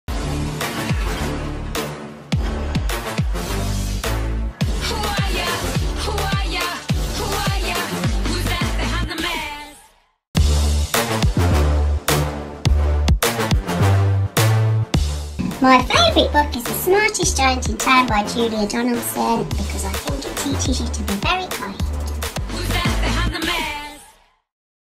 WBD 2020 | Masked Reader | Clue 1 Daisy
📚| WORLD BOOK DAY 2020 | Clue 1 Queenbee

We have lots going on through the week as part of @WorldBookDayUK on Thursday.

For a bit of fun welcome to the maskedreader

You'll see all of the Clue 1's today leading to the reveals on worldbookday

Who's behind Daisy 's mask?